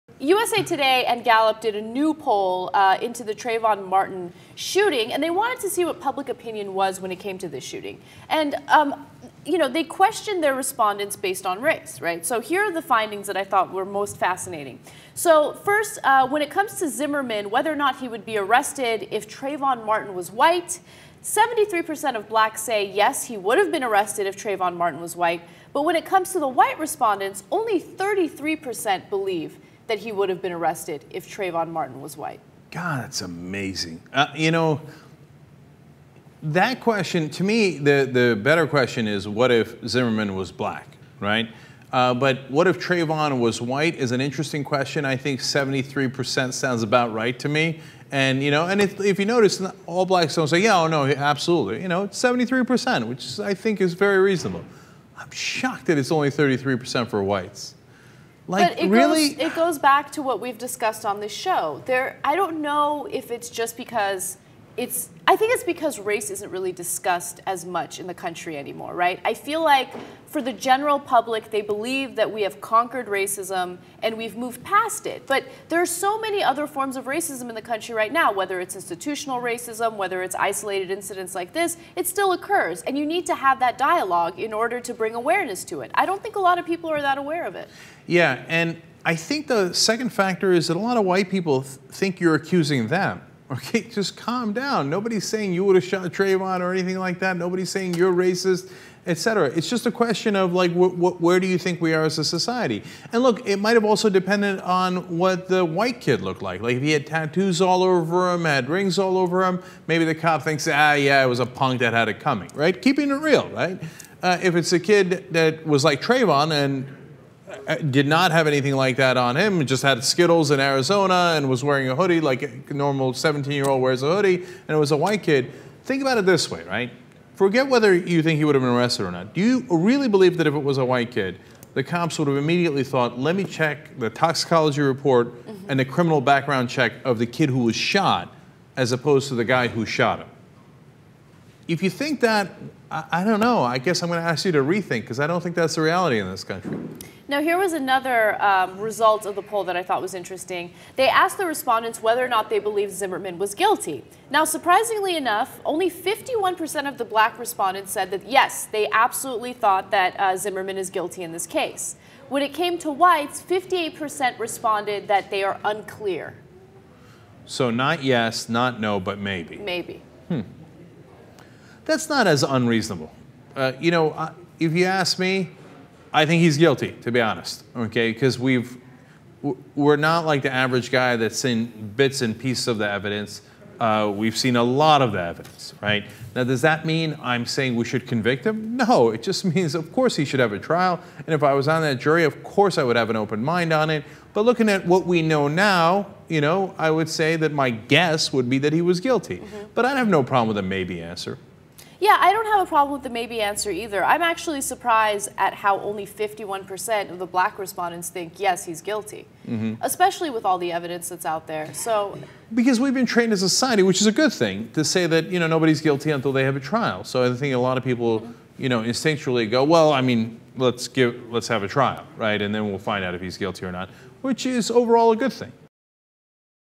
Trayvon Martin Shooting Poll Results
"USA Today and Gallup did a new poll into the Trayvon Martin shooting and they wanted to see what public opinion was when it came to the shooting..." Ana Kasparian and Cenk Uygur discuss the surprising results to the questions asked in the poll.